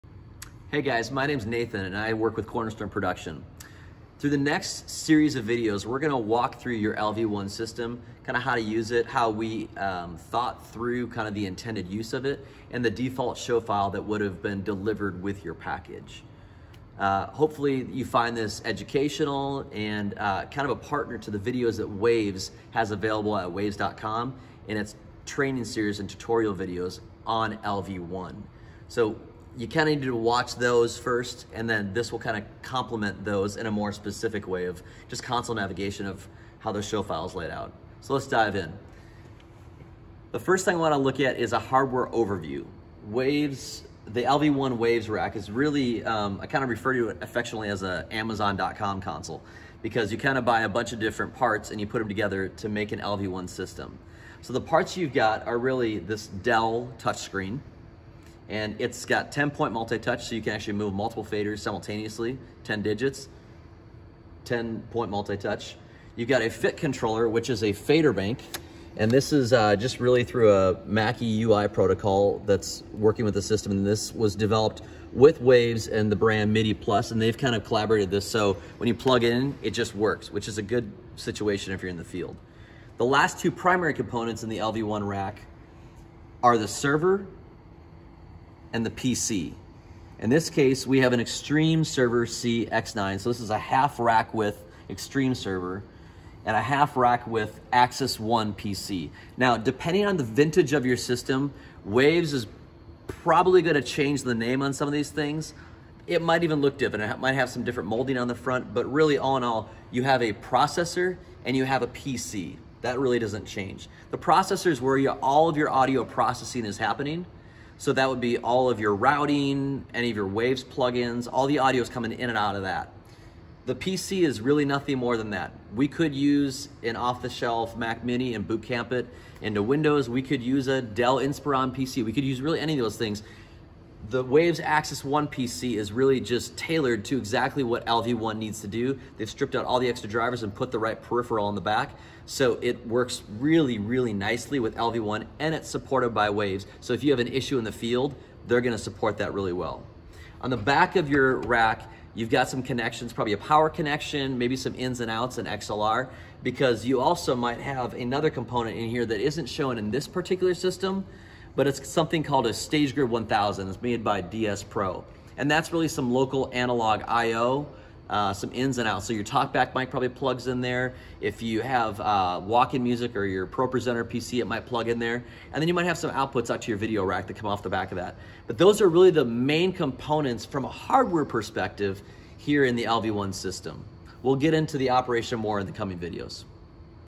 01 - LV1 Intro and Hardware Overview
An introduction to and hardware overview of the Salt Network LV1 mixing system.